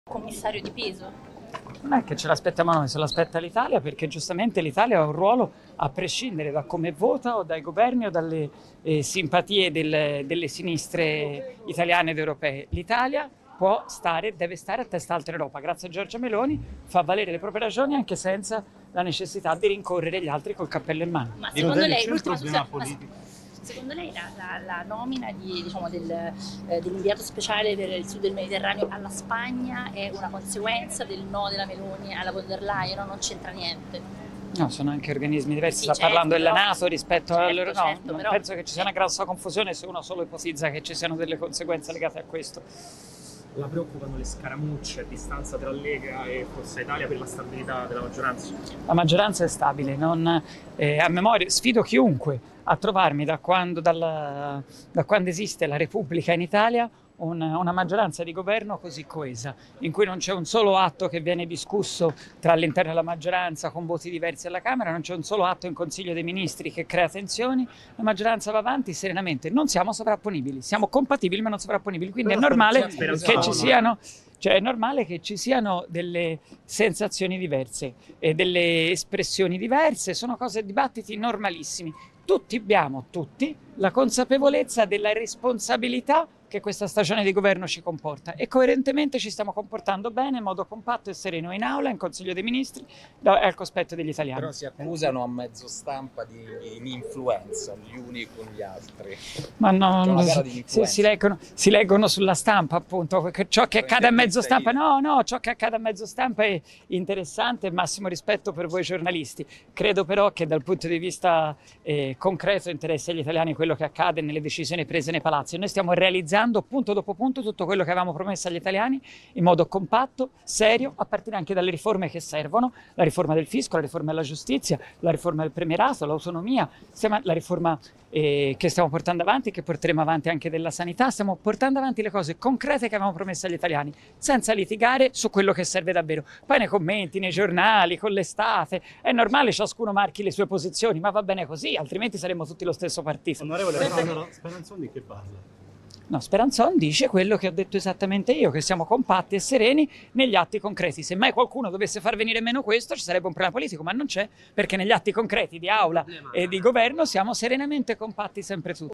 Governo, Donzelli: "Maggioranza coesa, mai così nella storia repubblicana"
"Speranzon dice quello che dico io, noi andiamo avanti con le nostre riforme"

Politica (Roma). "Se ci aspettiamo un commissario di peso in Europa? Non noi ma l'Italia perché il nostro Paese ha un ruolo importante e deve stare a testa in alta". Così il deputato di FdI, Giovanni Donzelli, intercettato fuori Montecitorio. "La maggioranza è stabile e sfido chiunque a trovarmene una così coesa nella storia della Repubblica    ha continuato parlando dello scontro tra Forza Italia e Lega -. Le accuse reciproche si leggono solo sulla stampa. Noi, comunque, andiamo avanti con le nostre riforme che abbiamo promesso agli italiani. Speranzon dice quello che dico io".  (Davide Di Carlo/alanews)

Aspetta ma non se lo aspetta l'Italia perché giustamente L'Italia ha un ruolo a prescindere da come vota Oh dai governi o dalle simpatie del delle sinistre italiane di europei l'Italia può stare devi stare attenta altra roba Grazie Giorgia Meloni fa valere le proprie ragioni anche senza la necessità di rincorrere gli altri col cappello in mano speciale per la Spagna è una conseguenza del nome della meloni alla io non c'entra niente No sono anche organismi rispetto alla confusione che ci siano delle conseguenze legate a questo  la maggioranza è stabile non ha memoria Sfido chiunque a trovarmi da quando esiste la Repubblica in Italia una maggioranza di governo così coesa in cui non c'è un solo atto che viene discusso tra le interne alla maggioranza con voci diverse la camera non c'è un solo atto in consiglio dei ministri che crea la maggioranza va avanti serenamente non siamo sopra ponibili siamo compatibili meno sovrapponibili Quindi è normale che ci siano cioè normale che ci siano delle sensazioni diverse e delle pressioni diverse sono cose di battiti normalissimi tutti abbiamo tutti la consapevolezza della responsabilità che questa stagione di governo si comporta e coerentemente ci stiamo comportando bene modo compatto sereno in aula In Consiglio dei Ministri è al cospetto degli italiani sulla Stampa  che ciò che accade a mezzo stampa No no ciò che accade a mezzo stampa è interessante massimo rispetto per voi giornalisti Credo però che dal punto di vista concreto interesse gli italiani quello che accade nelle decisioni prese stiamo realizzando punto dopo punto tutto quello che abbiamo promesso agli italiani in modo compatto serio a partire anche dalle riforme che servono la riforma del fisco riforma della Giustizia la riforma il premier ha solo sonno mi ha informato che stiamo portando avanti che porteremo avanti anche della sanità stiamo portando avanti le cose concrete che avevo promesso gli italiani senza litigare su quello che sarebbe davvero Poi nei commenti nei giornali con l'estate le sue posizioni ma va bene così altrimenti saremmo tutti lo stesso Partito amore volevo quello che ha detto esattamente io che siamo compatti e sereni negli atti concreti mai qualcuno dovesse far venire meno questo sarebbe un politico ma non c'è perché negli atti concreti di aula e di governo siamo serenamente compatti Sam